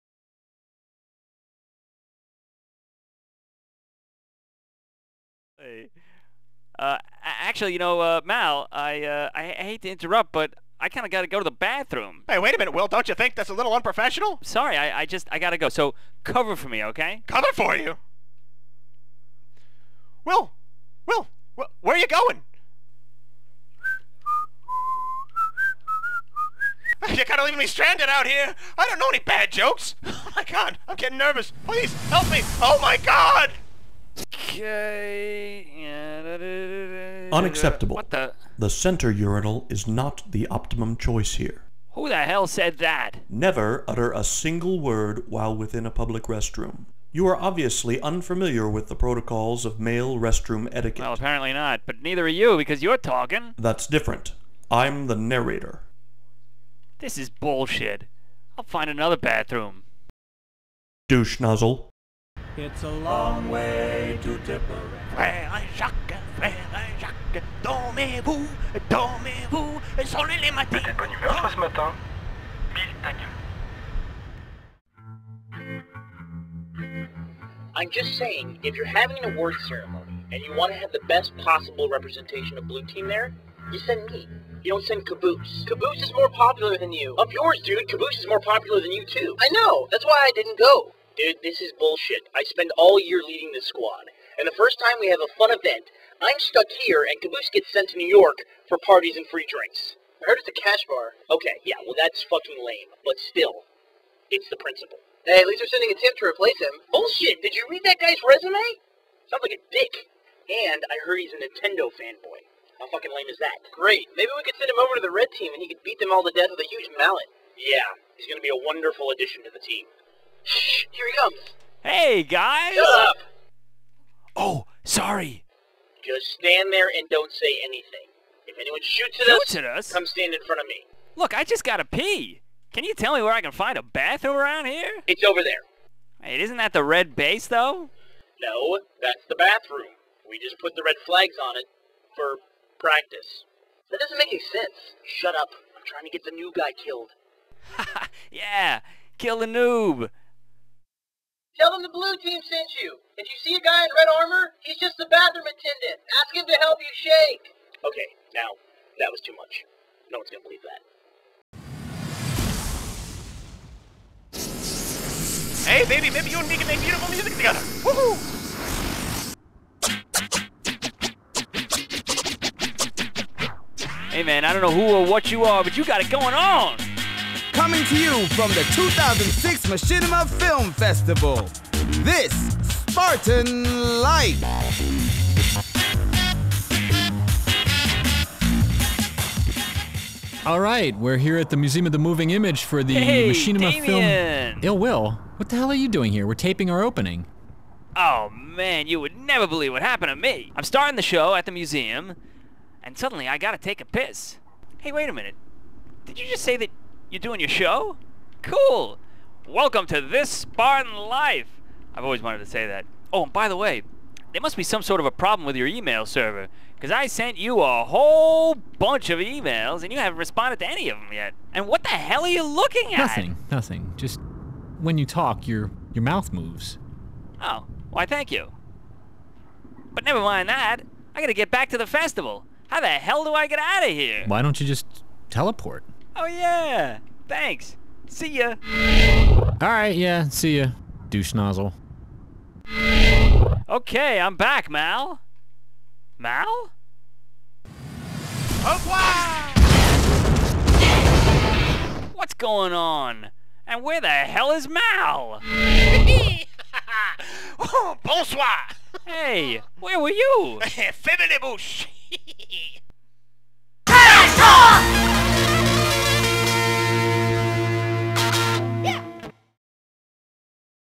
TrashTa1k at the 2006 Machinima Festival
ILL Will goes on an Oscars-style journey through the movies of some of the nominees of the 2006 Machinima Awards.